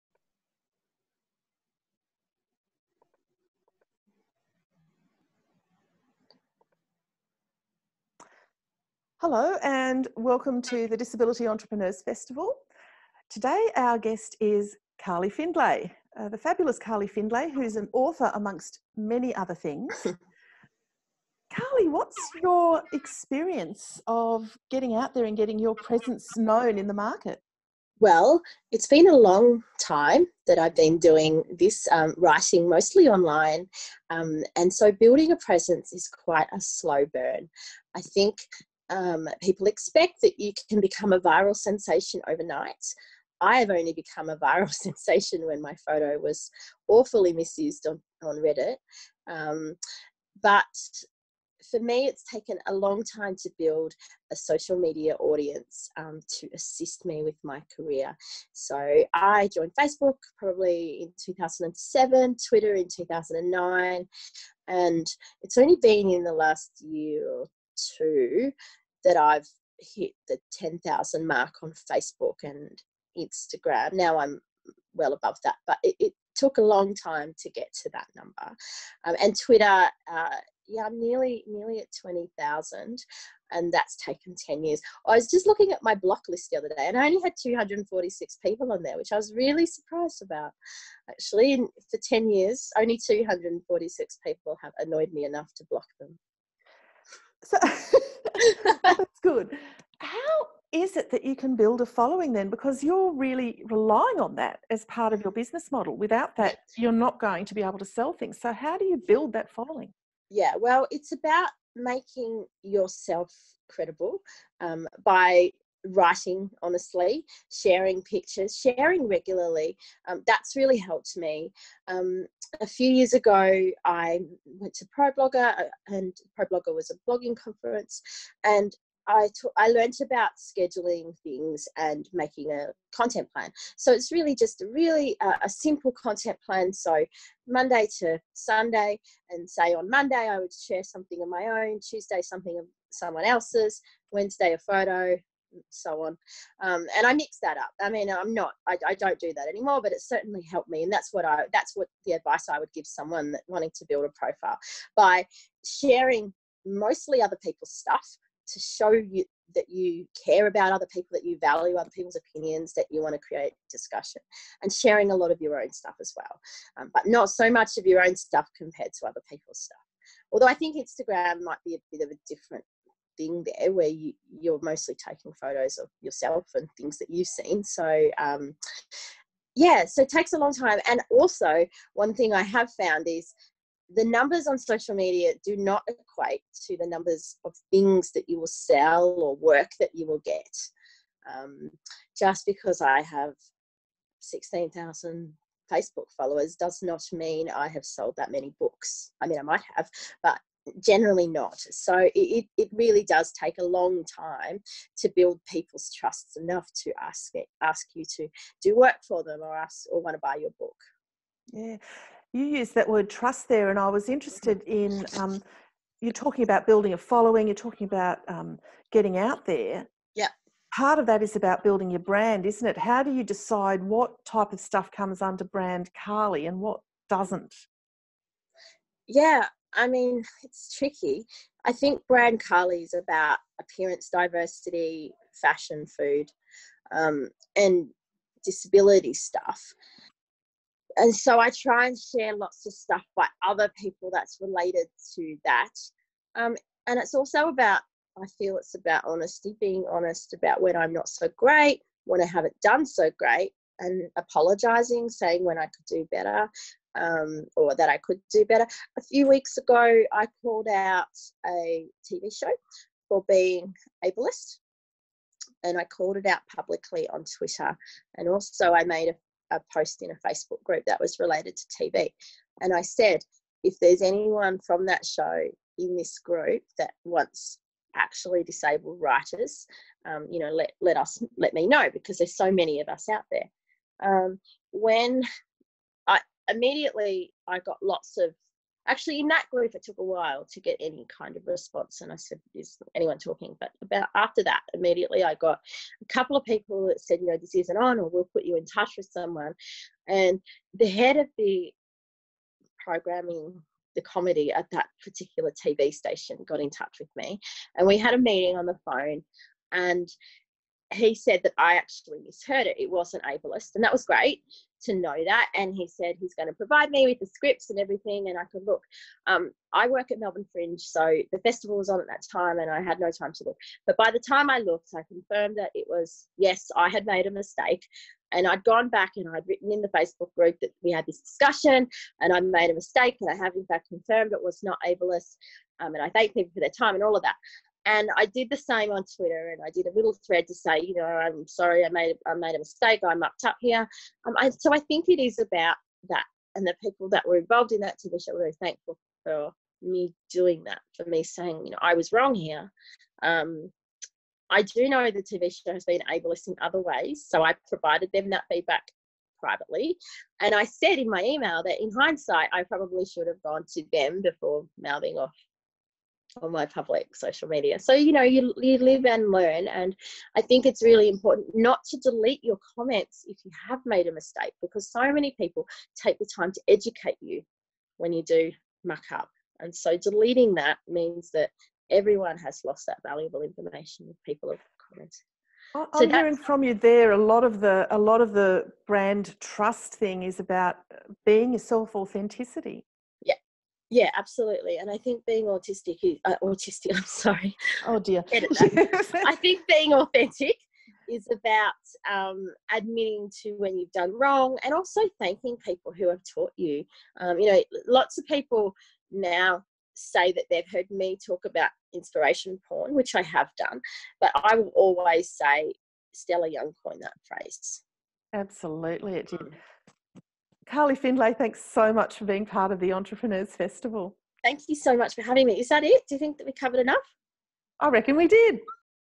Showcase: Carly Findlay
Carly outlines her branding, how she has built a following, and working to stay in her niche.